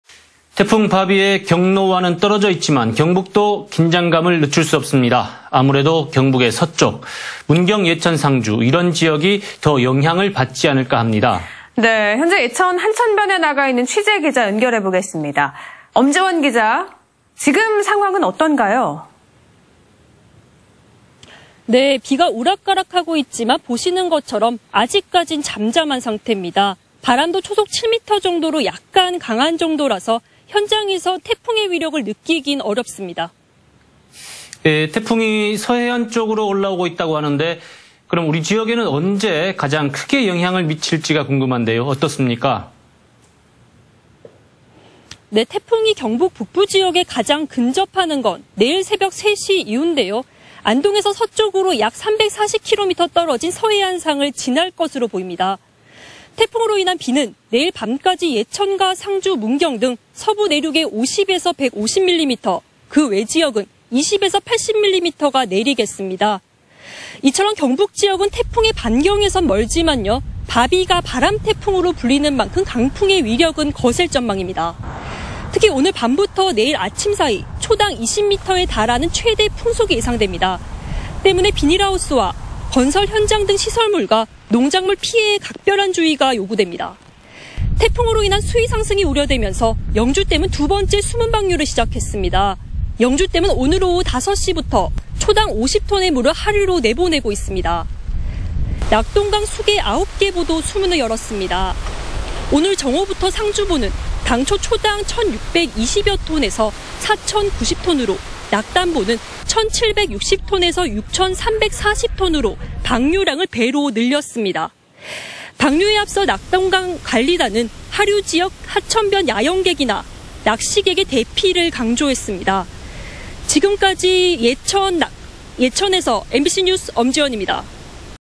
R-LTE] 태풍 '바비' 북상.. 이 시각 경북 / 안동MBC
2020/08/26 16:48:13    작성자 : 엄지원

◀ＡＮＣ▶
남]태풍 '바비'의 경로와는 떨어져 있지만
경북도 긴장감을 늦출 수 없습니다.
아무래도 경북의 서쪽,
문경 예천 상주 이런 지역이
더 영향을 받을 것 같은데요.

여]
예천 한천변에 나가 있는 취재기자 연결합니다. 엄지원 기자.
화면이 보니 바람이 심해진 것 같은데요?
◀ＥＮＤ▶
◀ＶＣＲ▶

기자]네. 오후들면서 바람이 조금씩 거세지더니 지금은 초속 -미터로
제법 강하게 불고 있습니다.

제가 몸을 가누기 힘들 정도는
아니지만...
태풍의 위력을 실감케 합니다.

앵커]네. 태풍이 서해안 쪽으로
올라오고 있다고 하는데 우리 지역엔 언제
가장 크게 영향을 미칠지 궁금한데요.
어떻습니까?

[기자] 태풍이 경북 북부지역에
가장 근접하는 건 내일 새벽 3시 이후인데요.

안동에서 서쪽으로 약 340km 떨어진
서해안을 지날 것으로 보입니다.

태풍으로 인한 비는
내일 밤까지 예천과 상주, 문경 등
경북 서부내륙에 50에서 150mm,
그외 지역은 20에서 80mm가 내리겠습니다.

V C R

경북 지역은 태풍의 반경에선 멀지만
바비가 '바람 태풍'으로 불리는 만큼,
강풍의 위력은 거셀 전망입니다.

특히 오늘 밤부터 내일 아침 사이
초당 20미터에 달하는 최대 풍속이 예상돼
비닐하우스와 건설현장 등의 시설물과,
농작물 피해에 각별한 주의가 필요합니다.

태풍으로 인한 수위 상승이 우려되면서
영주댐은 두 번째 수문방류를 시작했습니다.

영주댐은 오늘 오후 5시부터 수문을 열고
초당 50톤의 물을 하류로 보내고 있습니다.

낙동강 수계 9개 보도 수문을 열렸습니다.

오늘 정오부터 상주보는
당초 초당 1,62여톤에서 4천 90톤으로,
낙단보는 1,760톤에서 6천 340톤으로
방류량을 배로 늘려 내보내고 있습니다.

방류에 앞서 낙동강보 관리단은
하류지역 하천변 야영객이나 낚시객의
신속한 대피 등을 강조했습니다.

지금까지 예천 한천변에서
MBC뉴스 엄지원입니다. (영상취재 차영우)